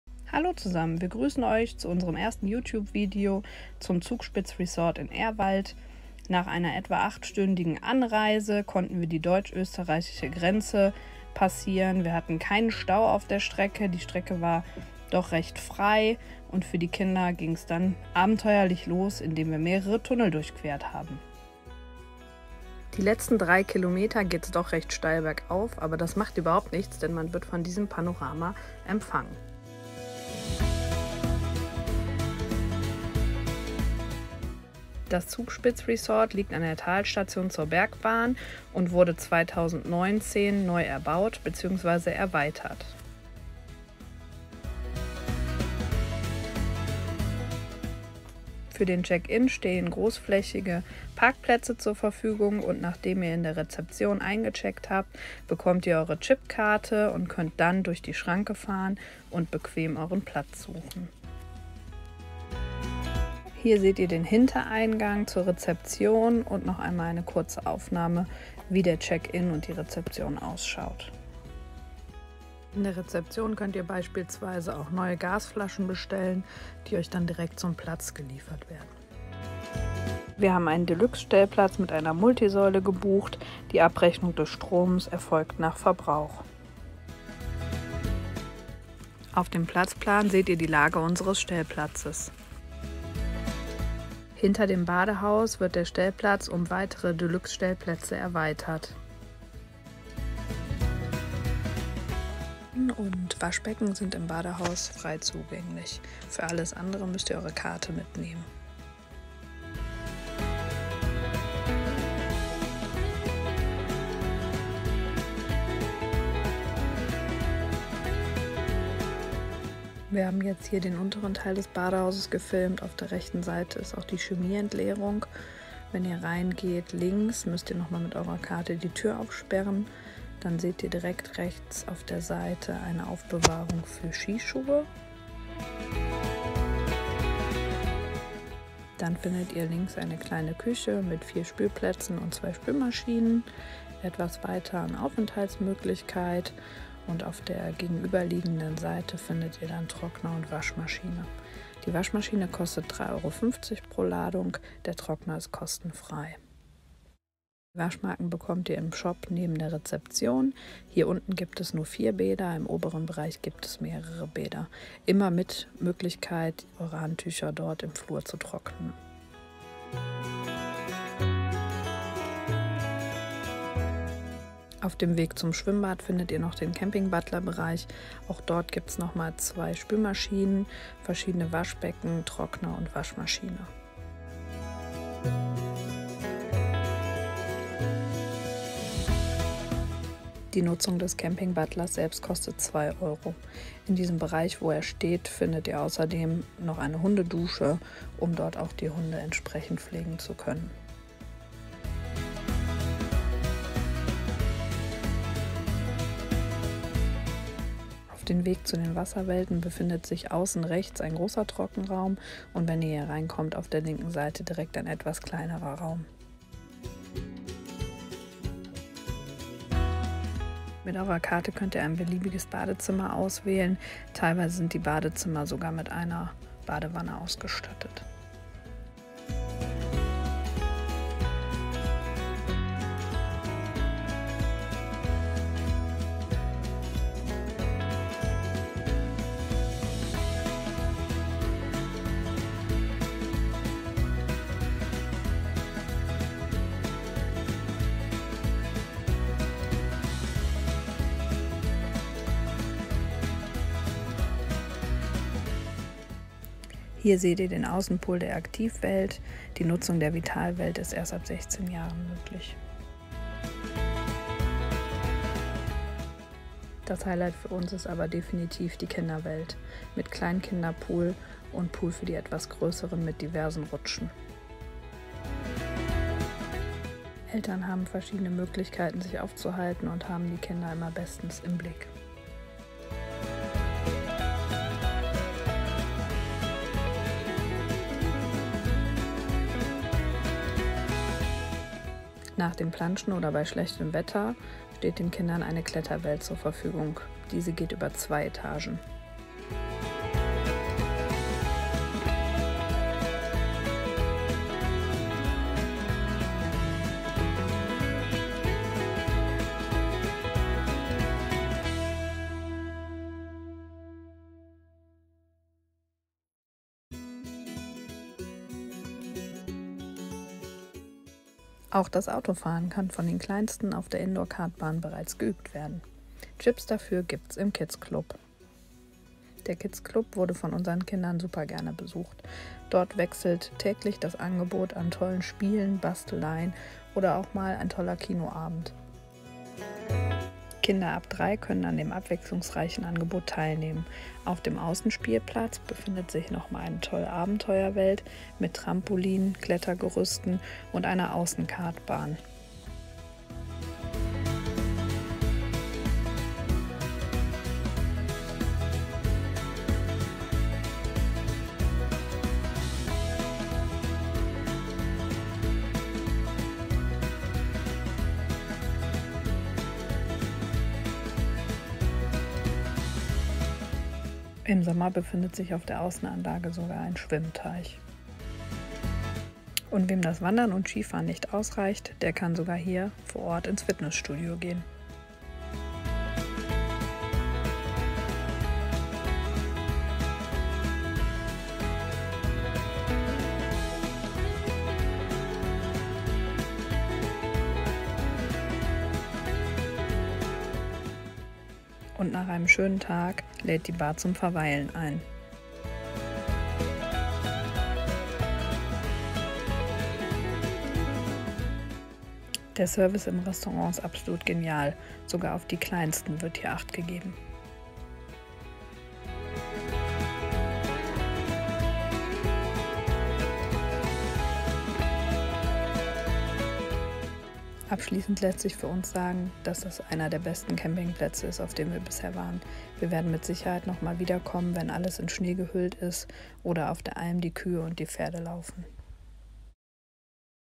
ZUGSPITZ Resort Ehrwald - Campingplatzvorstellung - mit uns durch Europa
In diesem Video möchten wir Euch den Campingplatz ZUGSPITZ Resort in Ehrwald vorstellen. Das Video wurde von uns aus freien Stücken und ohne Absprache erstellt. Trotzdem muss es als Werbung deklariert werden.